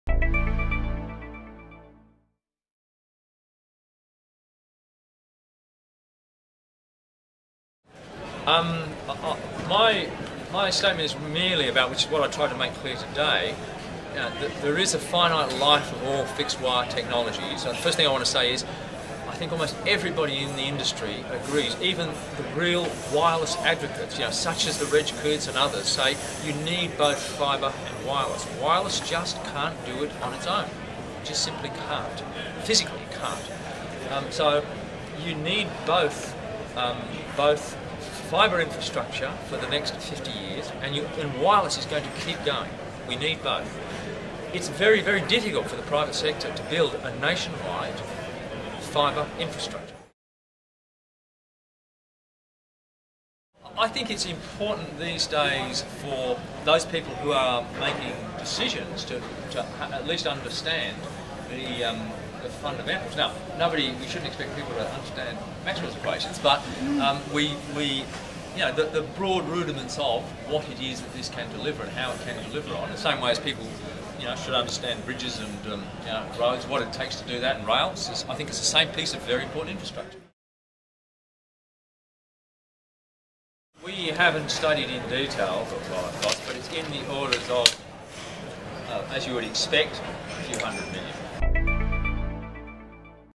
NBN CO. CEO: Q&A on Election Eve
Mike Quigley discusses the Coalition's broadband policy, 'tech heads' and what it would cost to disband NBN Co.